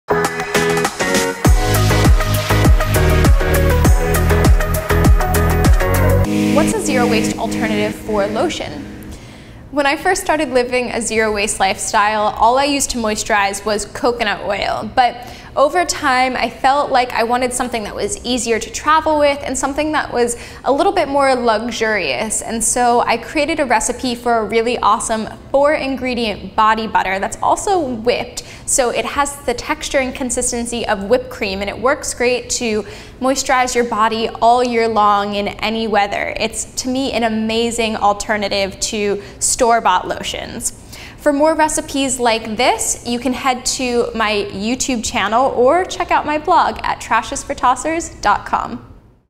FAQ: What do you use as Zero Waste Lotion?
In this video Lauren Singer from Trash is for Tossers will tell you what she uses as lotion in place of store bought, plastic packaged items.